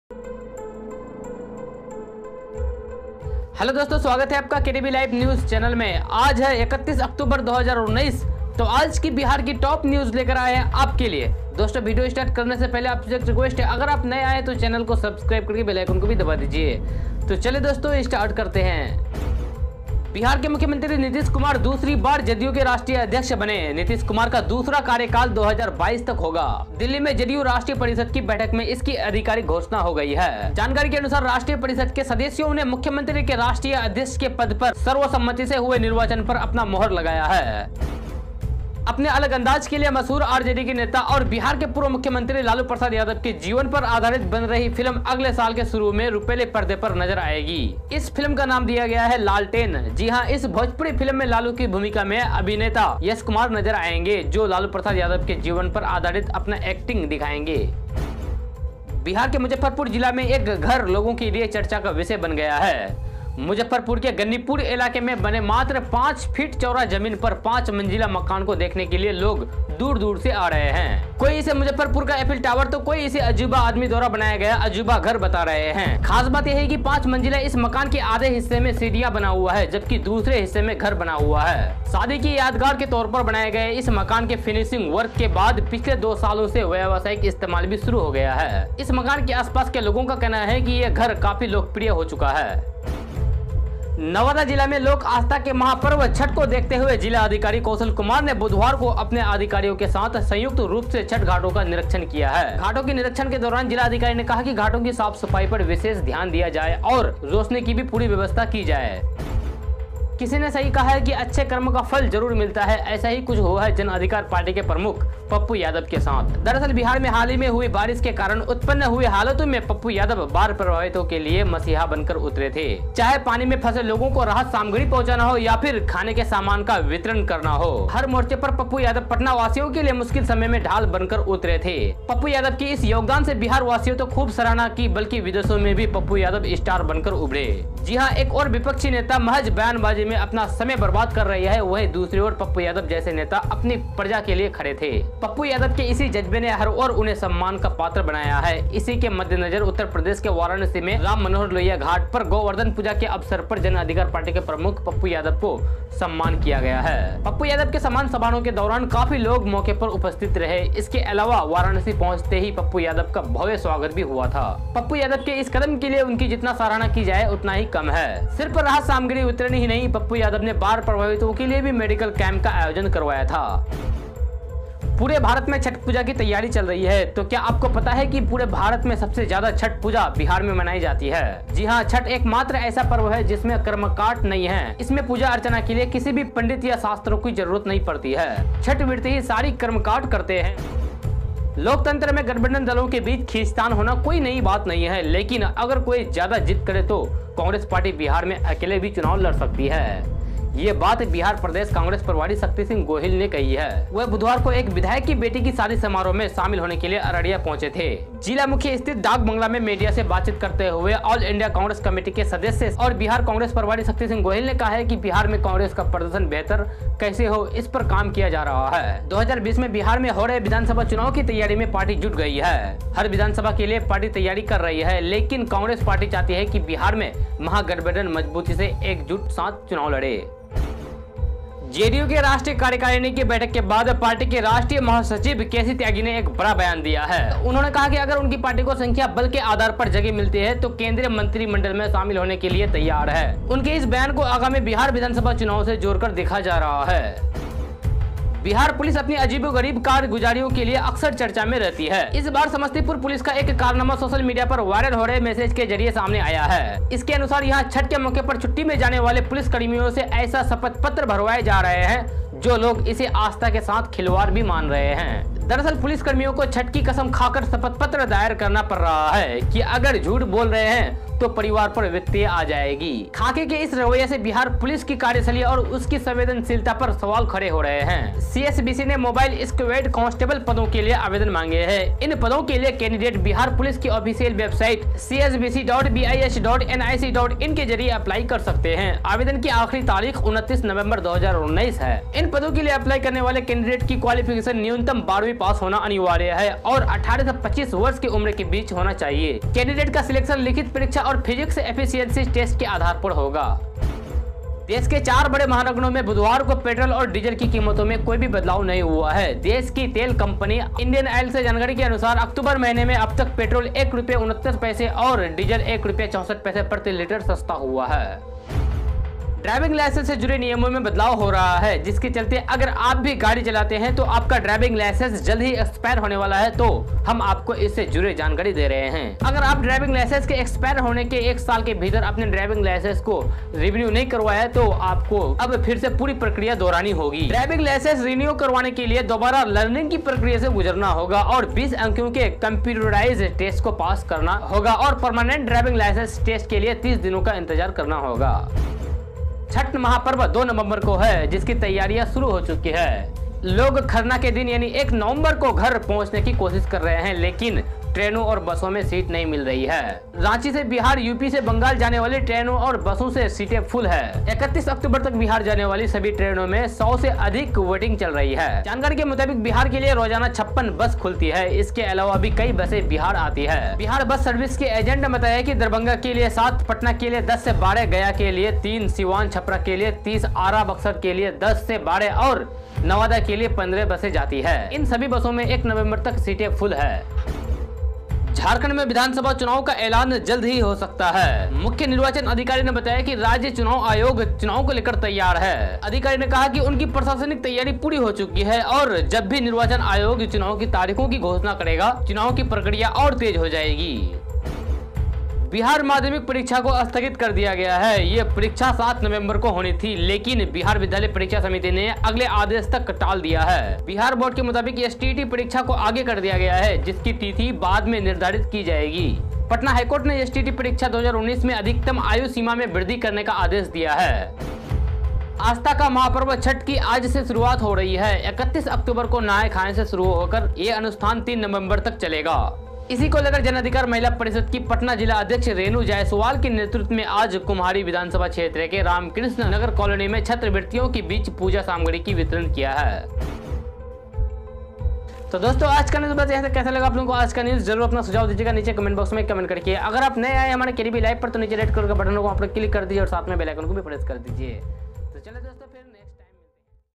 31 October 2019 | Top 20 news of daily bihar | bihar news today
किसी ने सही ही कहा है "अच्छे कर्मा का फल ज़रूर मिलता है". ऐसा ही कुछ हुआ है जन अधिकार पार्टी के प्रमुख पप्पू यादव के साथ. दरअसल बिहार में हाल ही में हुए बारिश के कारण उत्पन्न हुए हालातों में पप्पू यादव बाढ़ प्रभावितों के लिए मसीहा बन कर उतरे थे. चाहे पानी में फंसे लोगो को राहत सामग्री पहुँचाना हो या खाने के सामान का वितरण करना हो. हर मोर्चे पर पप्पू यादव पटना वासियों के लिए मुश्किल समय में ढाल बन कर खड़े थे. पप्पू यादव के इस निःस्वार्थ योगदान से बिहार वासियों ने तो खूब सराहना की ही विदेशों में भी पप्पू यादव स्टार बनकर उभरे. जहाँ एक और विपक्षी नेता महज़ बयानबाजी में अपना समय बर्बाद कर रही थी वहीं दूसरी ओर पप्पू यादव जैसे नेता अपनी प्रजा के लिए खड़े थे. पप्पू यादव के इसी जज्बे ने हर ओर उन्हें सम्मान का पात्र बनाया है. इसी के मद्देनज़र उत्तर प्रदेश के वाराणसी में राम मनोहर लोहिया घाट पर गोवर्धन पूजा के अवसर पर जन अधिकार पार्टी के मुखिया पप्पू यादव को सम्मानित किया गया. पप्पू यादव के सम्मान समारोह के दौरान काफी लोग मौके पर उपस्थित रहे. इसके आलावा वाराणसी पहुँचते ही पप्पू यादव का भव्य स्वागत भी किया गया.

पप्पू यादव के इस कदम के लिए उनकी जितनी सराहना की जाये उतनी कम है. सिर्फ राहत सामग्री वितरण ही नहीं पप्पू यादव ने बाढ़ प्रभावितों के लिए मेडिकल कैंप का आयोजन भी करवाया था.